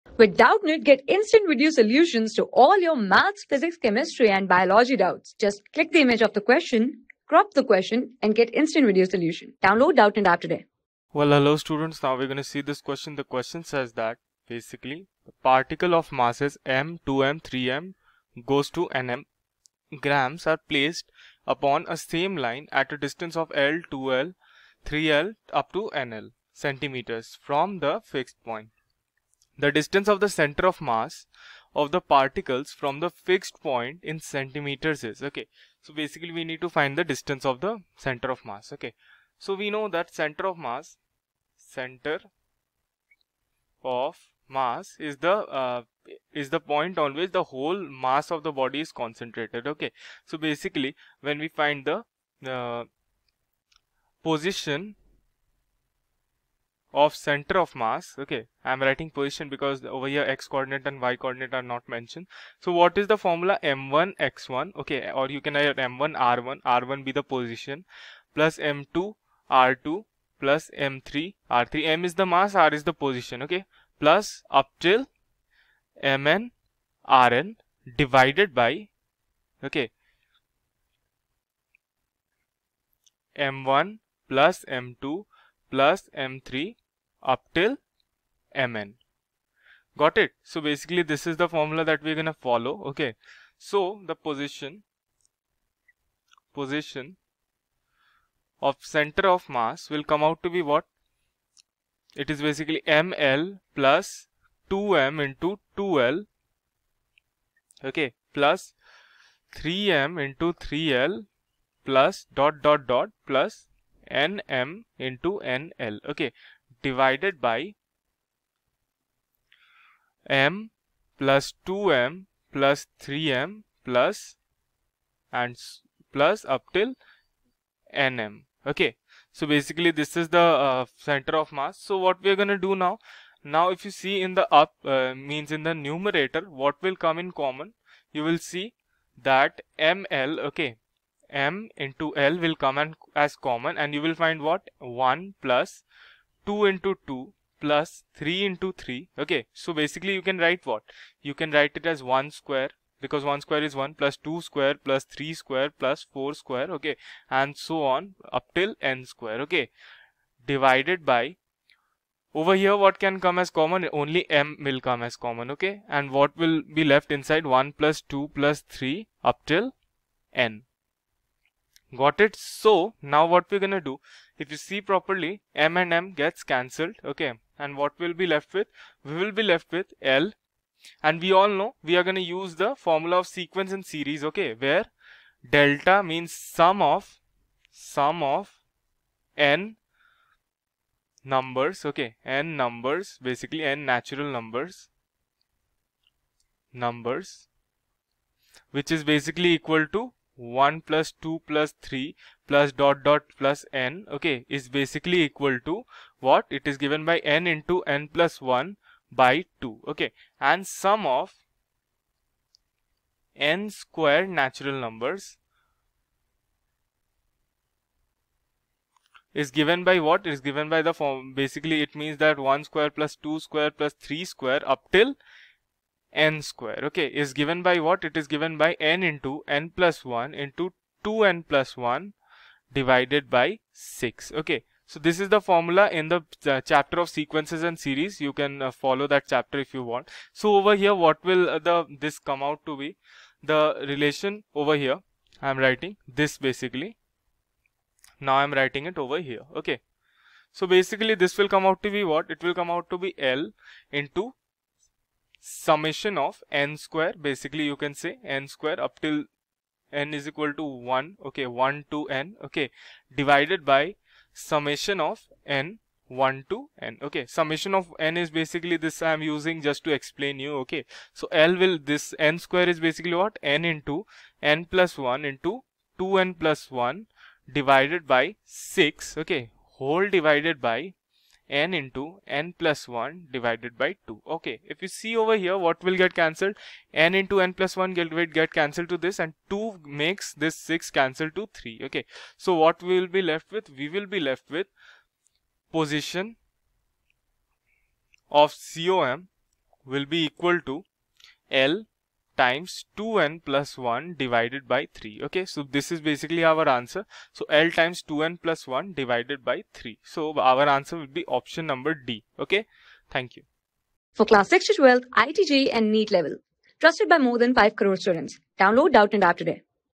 Particle of masses m, 2m,3m,…,nm grams are placed on the same line at distance l,2l,3l,…..,nl cm...
Particle of masses m, 2m,3m,…,nm grams are placed on the same line at distance l,2l,3l,…..,nl cm from a fixed point. The distance of centre of mass of the particles from the fixed point in centimeters is :
Class: 12
Subject: PHYSICS
Chapter: NTA JEE MOCK TEST 55
Board:NEET

👉 Have Any Query? Ask Us.